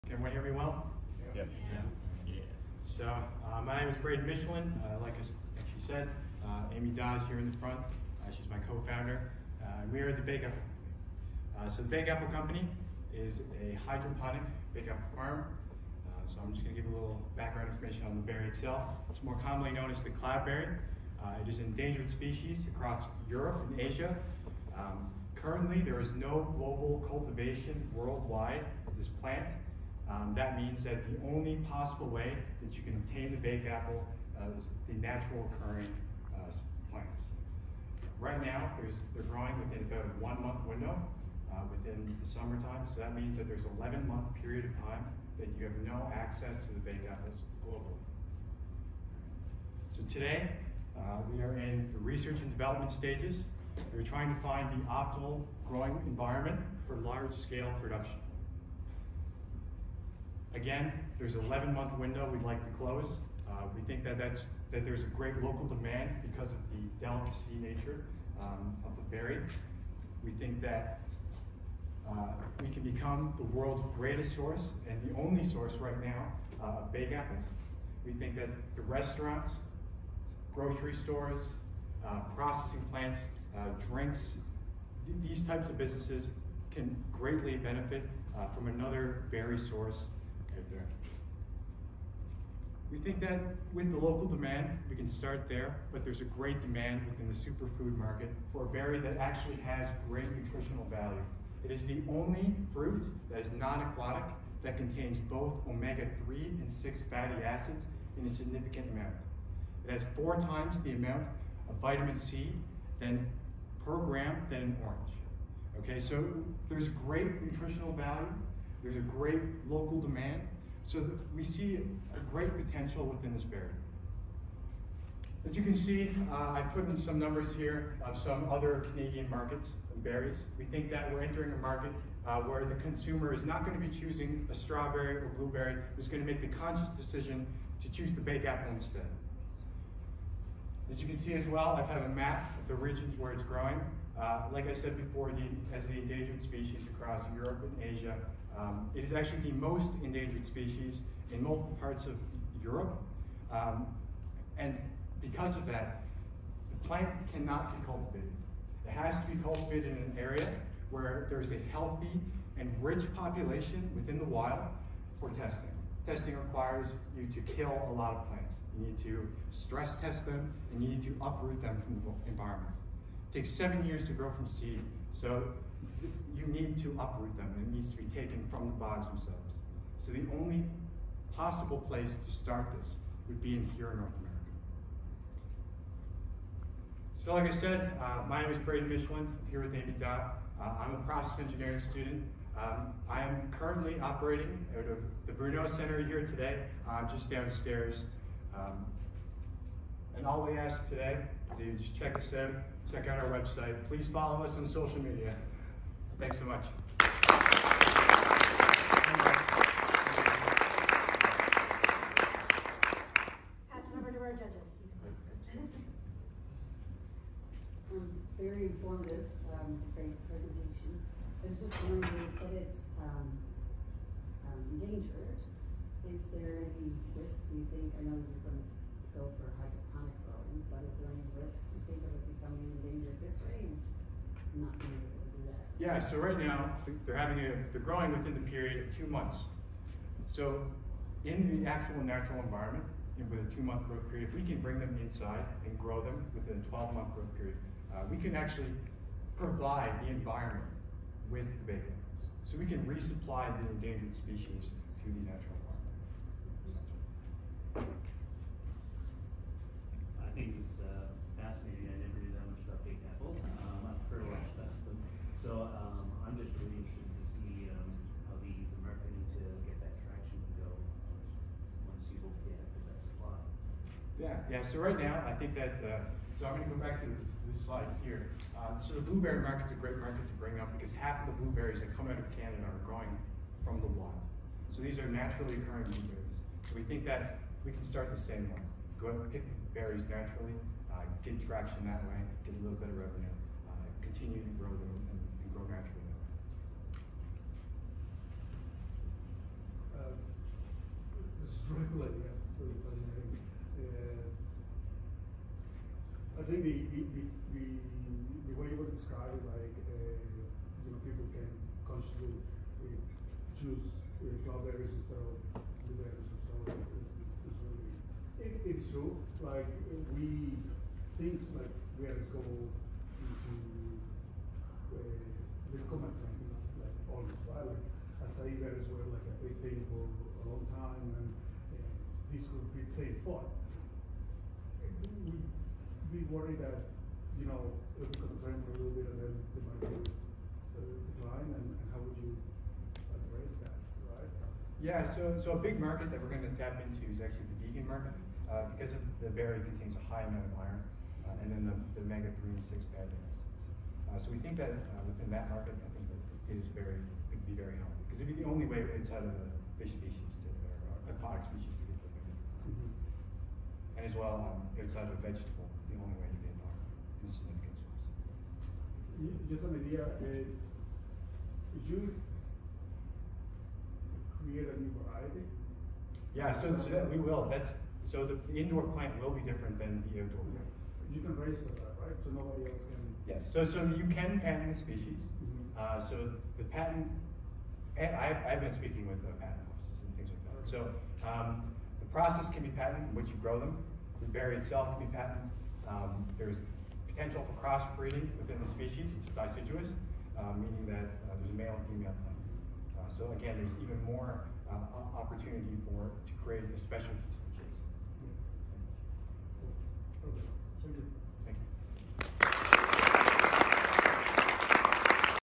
The Bakeapple Company Live Pitch 2023
During the 2023 winter Genesis program we had the oportunity to share our initial business pitch.